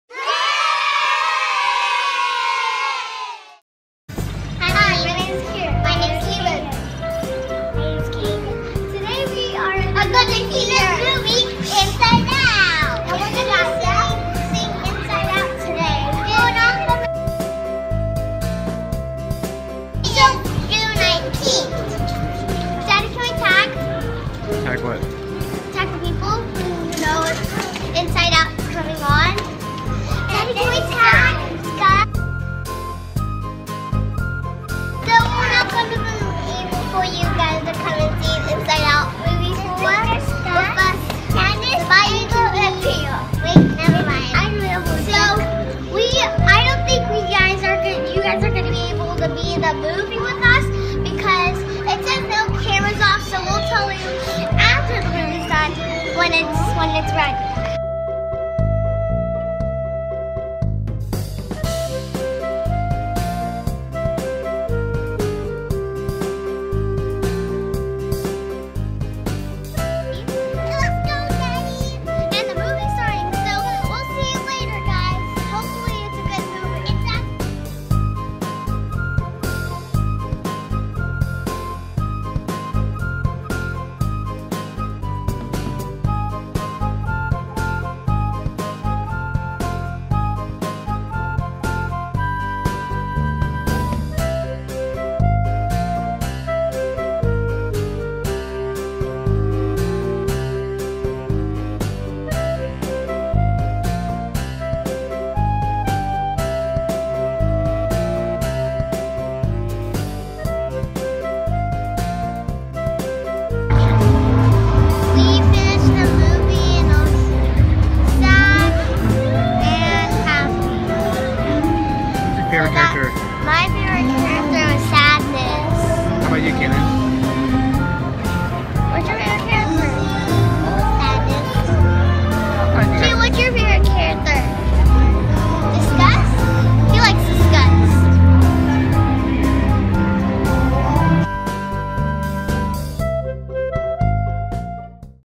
Inside Out Movie viewing on opening day
Kakeiran go to the movies to watch Inside Out on opening day. The movie lobby was filled with cut outs of the upcoming movies such as the Peanuts, Good Dinosaur, Minions, and Hotel Transylvania. We were able to take pictures since it was a Friday afternoon and the theater crowd was pretty light. The previews were quite entertaining with the lineup mentioned above. There was also a short film called Lava that played to our hearts since we just got back from our summer vacation trip to Maui.  The movie was entertaining.